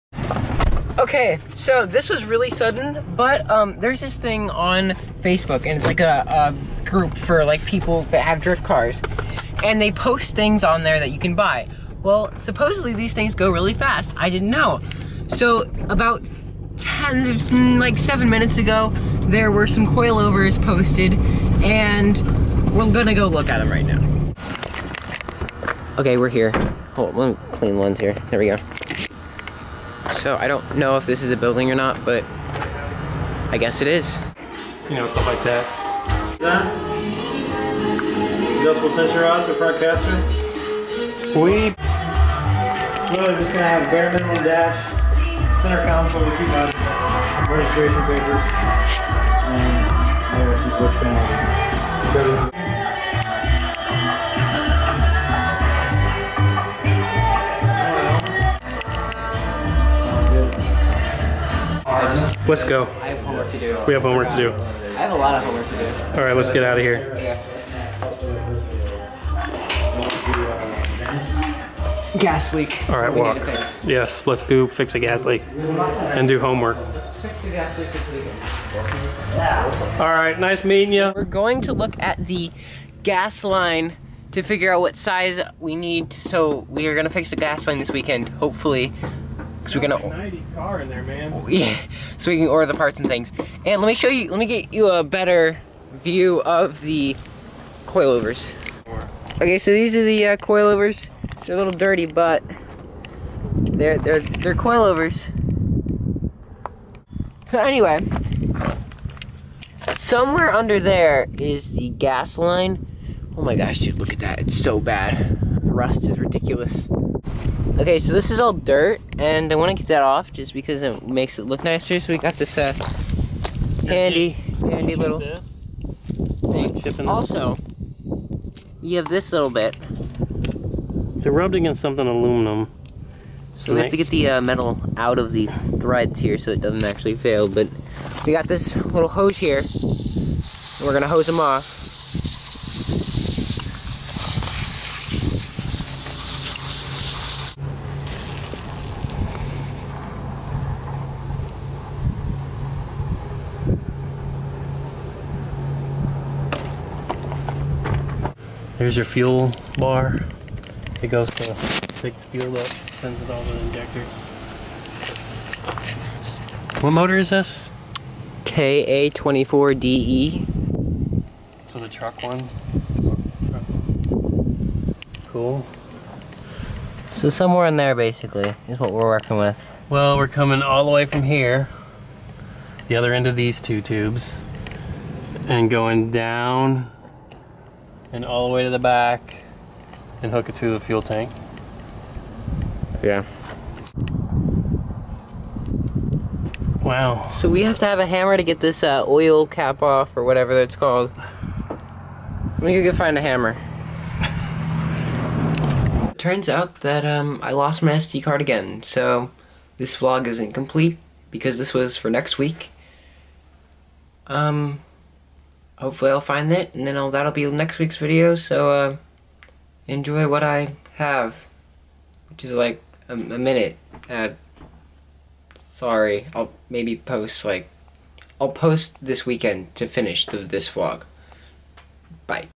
I Bought Coil Overs | vlog 26
my dad ang i go to look at some red coil overs.  but i lost my sd card so the vlog is really short.